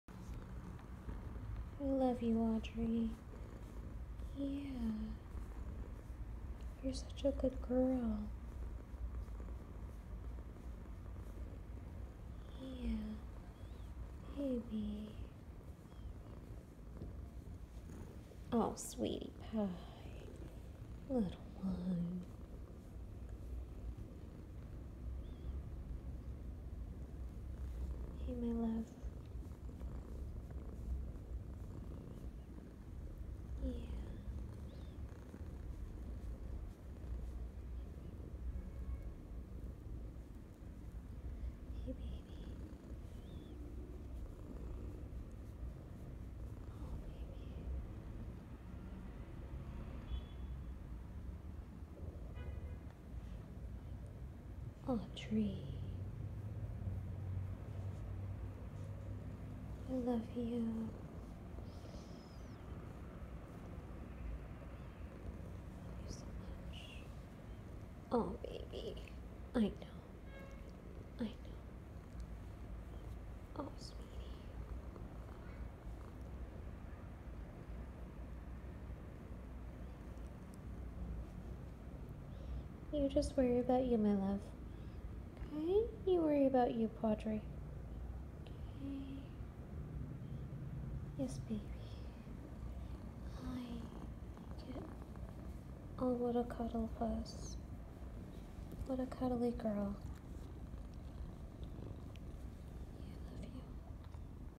Video of adoptable pet named Regal & Playful Audrey
Regal & Playful Audrey is an adoptable pet with Brooklyn Animal Action in Brooklyn NY.